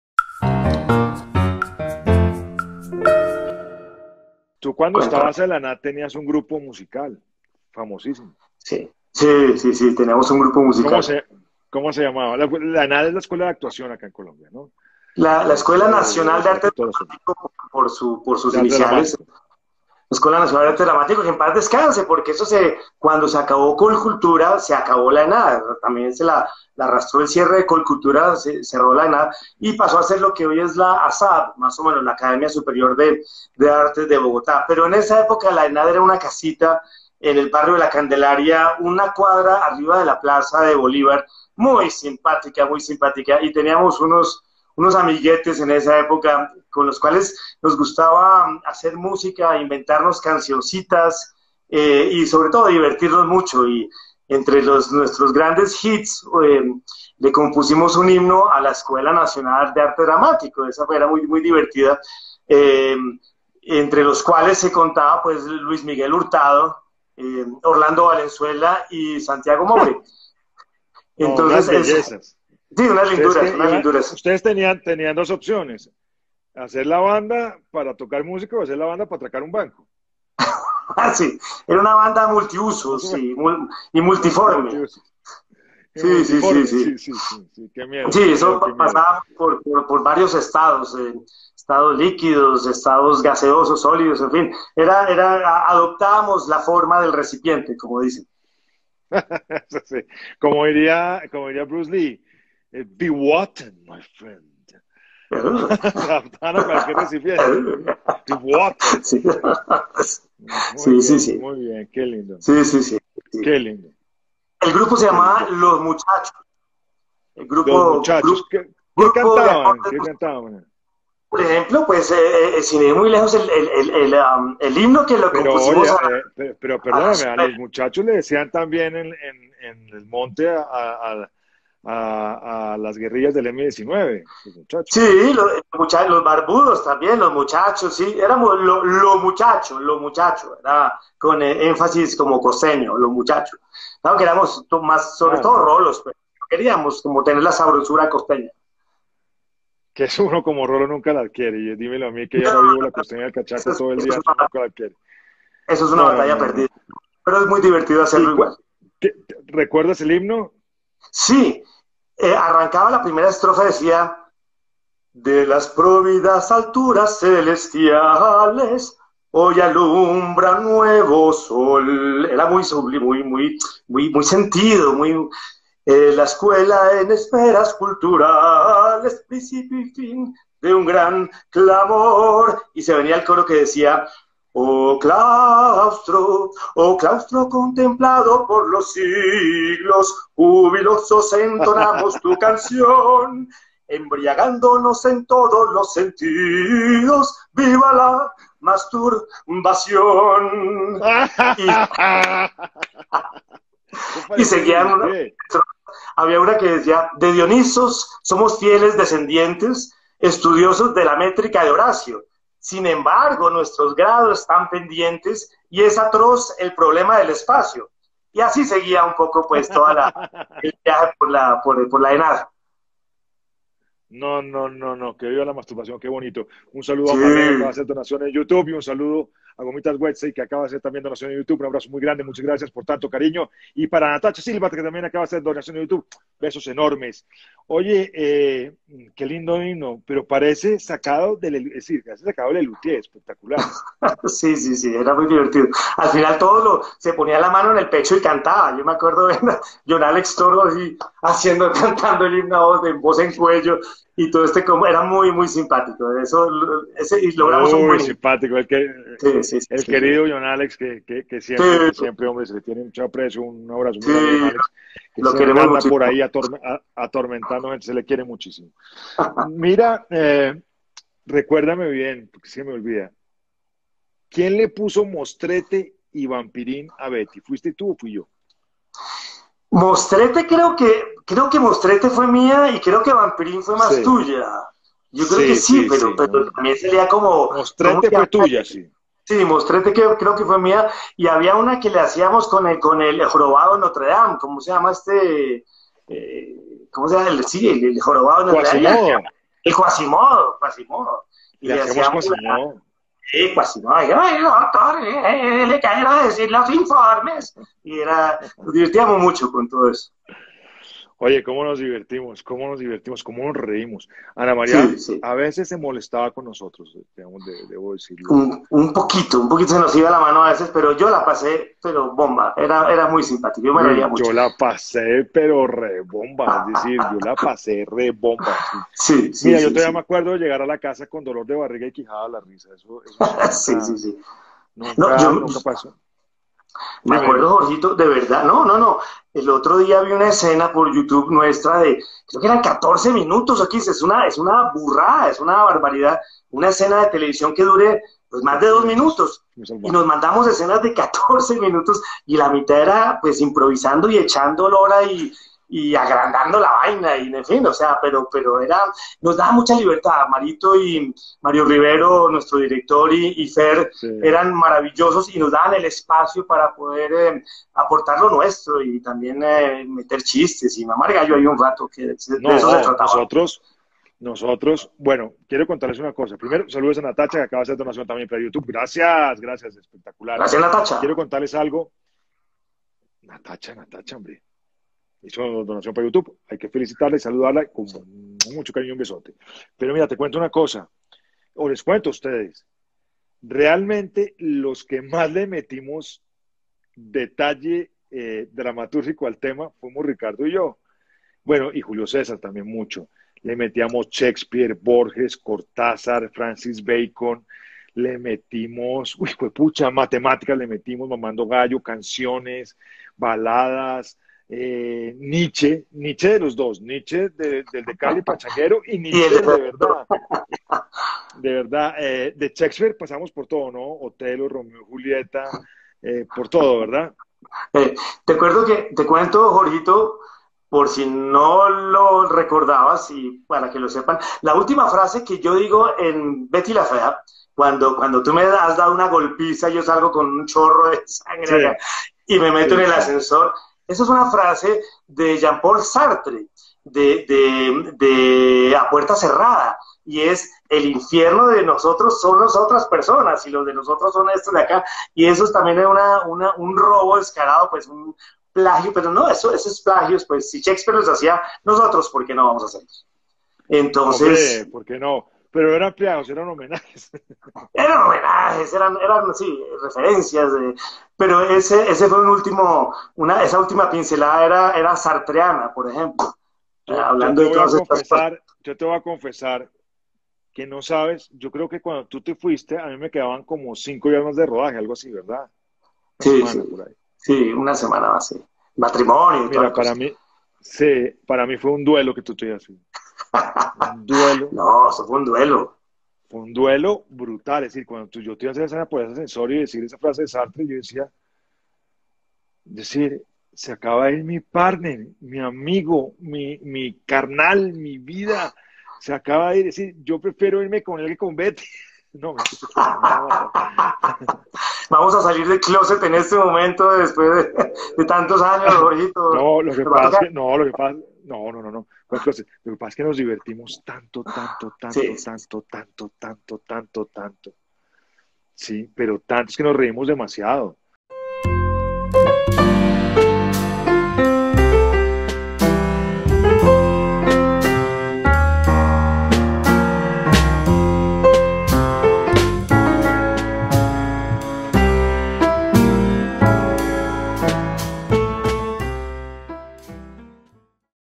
Especial de Betty la fea...Ricardo Vélez recuerda su personaje de Mario Calderón
Uno de mis grandes amigos y compañeros de set en BETTY LA FEA:  RICARDO VÉLEZ. Recordamos INOLVIDABLES MOMENTOS